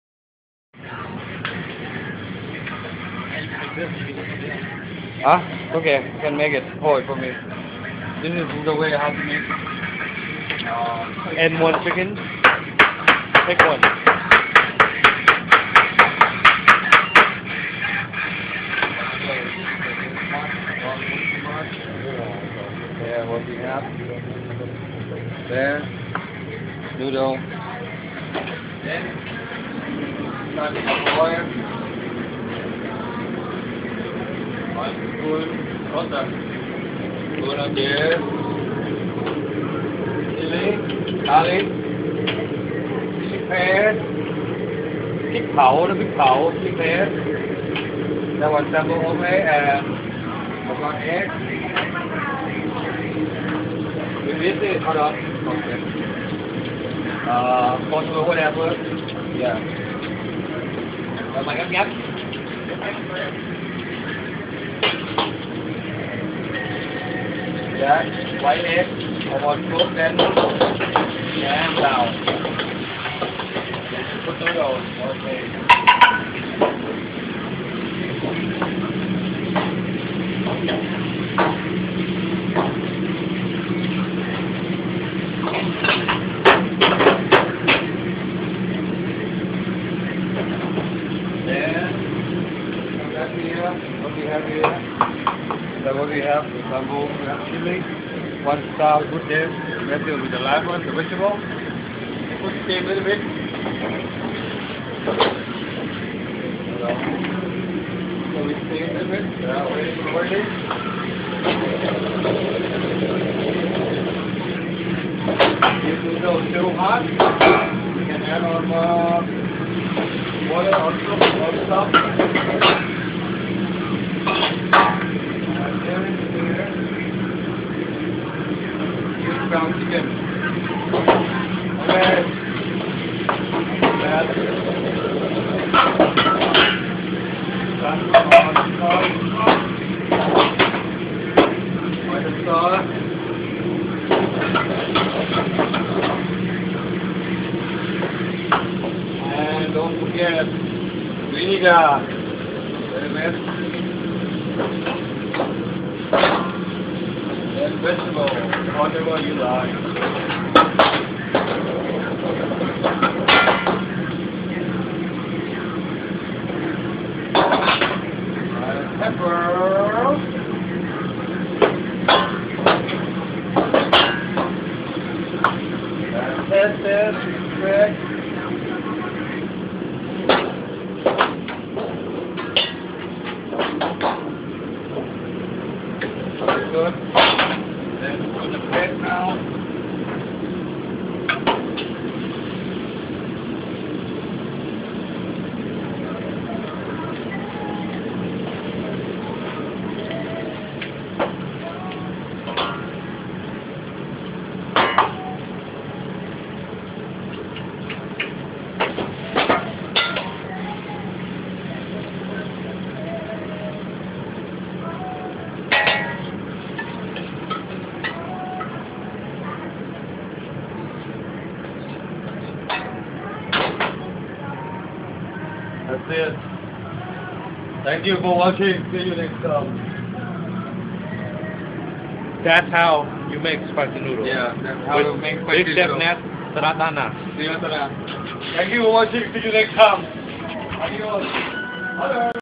How to cook Thai Style Basil Noodle (pad-kee-mow)
How to cook Thai Spicy Basil Noodle aka Pad Kee Mow or Drunken Noodle